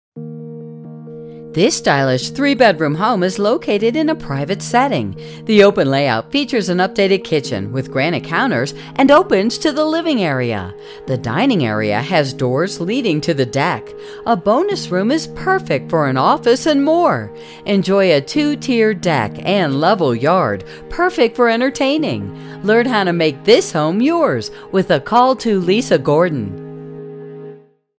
470 Birch Mountain Road Manchester, CT | ColdwellBankerHomes.com
470 Birch Mountain Road, Manchester, CT

Stylish and unique layout is open and airy plus located in a private setting! This home has it all!  The kitchen has been updated with granite countertops, stainless steel appliances and is open to the main living area.  Dining area has an ornate tile floor and doors leading to the deck. Open to the